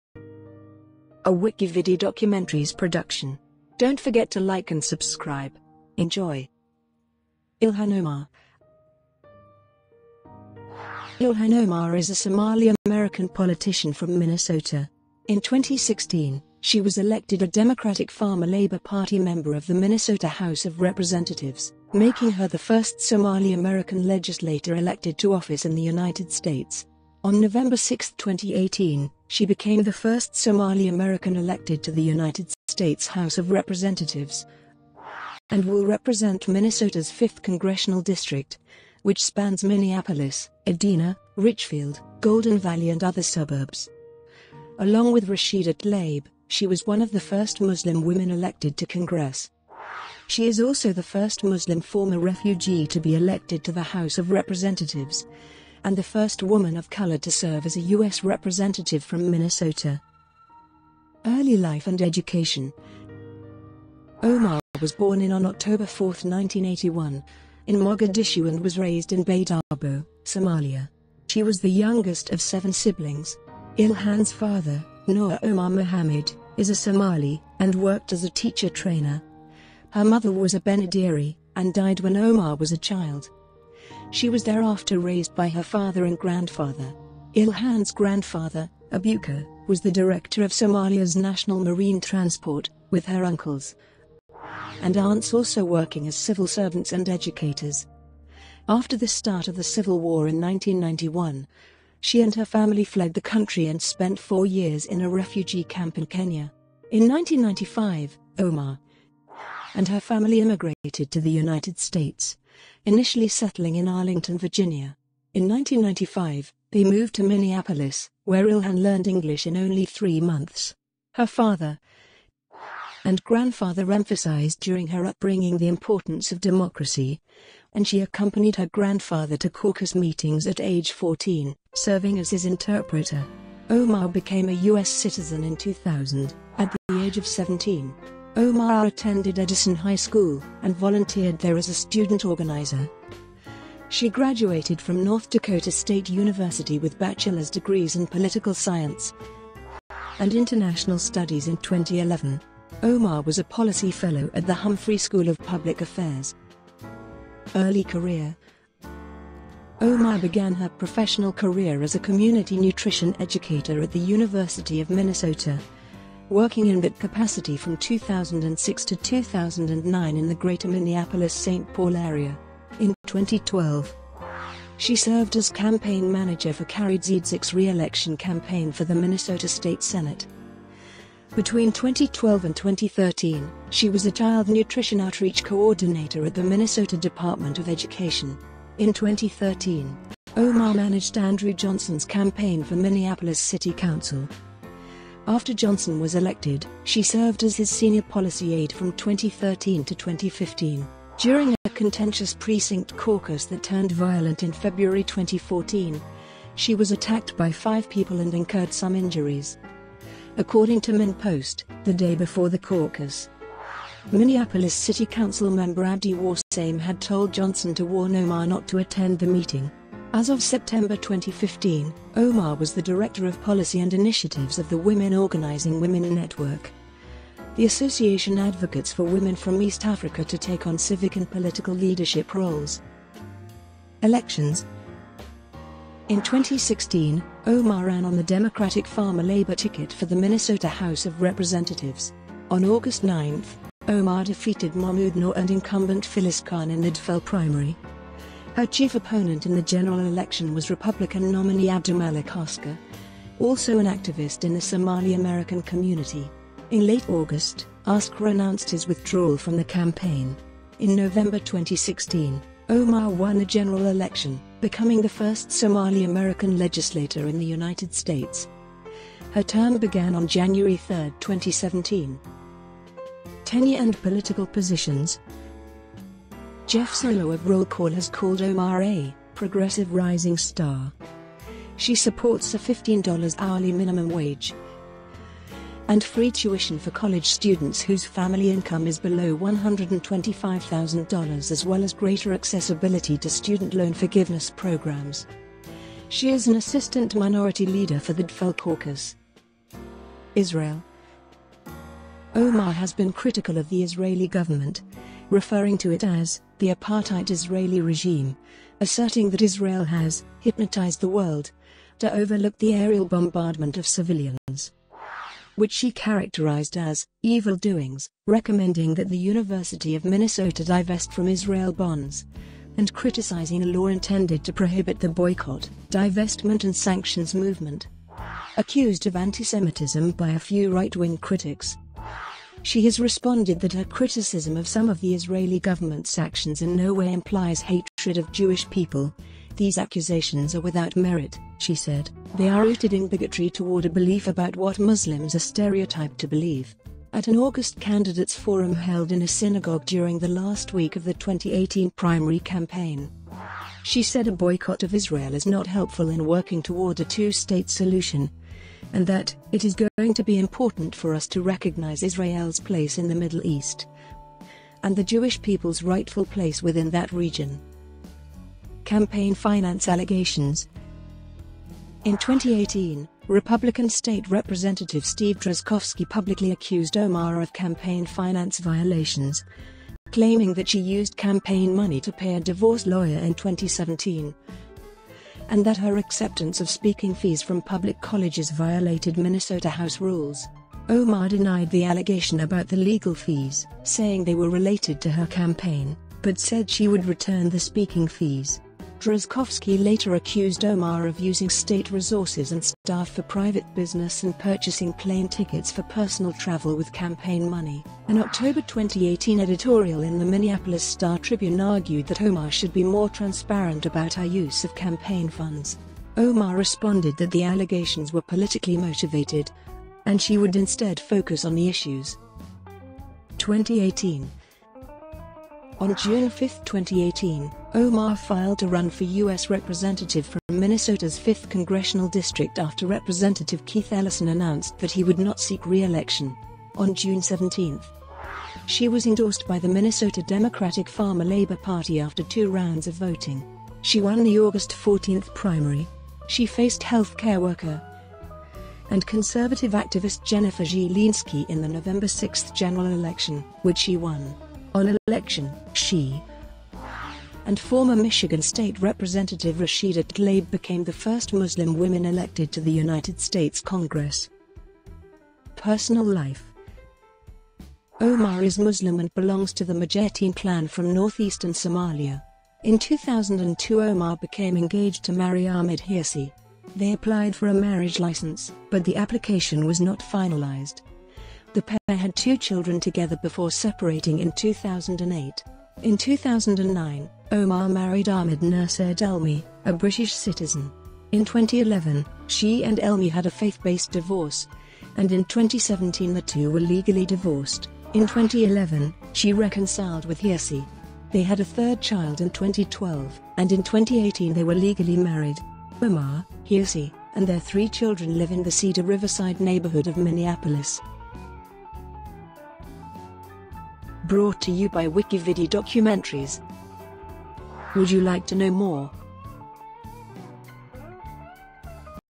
ILHAN OMAR - WikiVidi Documentary
Ilhan Omar is a Somali American politician from Minnesota. In 2016, she was elected a Democratic–Farmer–Labor Party member of the Minnesota House of Representatives, making her the first Somali American legislator elected to office in the United States. On November 6, 2018, she became the first Somali American elected to the United States House of Representatives and will represent Minnesota's 5th congressional district, which spans Minneapolis, Edina, Richfield, Golden Valley and other suburbs. Along with Rashida Tlaib , she was one of the first Muslim women elected to Congress. She is also the first Muslim former refugee to be elected to the House of Representatives, and the first woman of color to serve as a U.S. Representative from Minnesota.

Shortcuts to chapters:
00:01:01 Early life and education
00:02:37 Early career
00:03:53 Elections
00:04:33 Tenure and political positions
00:05:00 Israel
00:06:08  Campaign finance allegations
00:07:03 2018

Copyright WikiVidi.